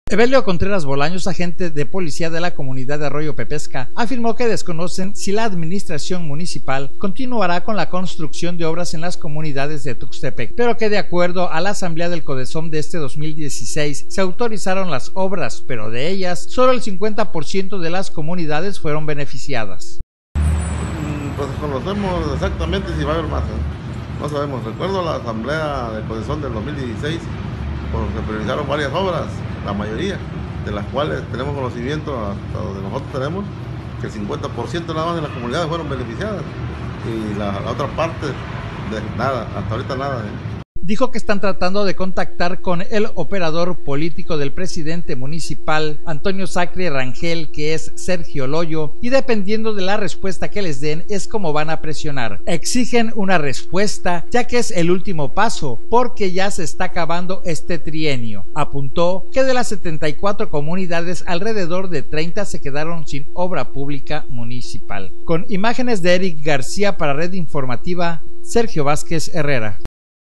Ayuntamiento no cumplió con obras del CODESOM: Agentes
-Alrededor de 30 comunidades se quedaron sin obra pública: Evelio Contreras Bolaños

Descarga nuestra APP para Android desde y sigue todas nuestras actualizaciones.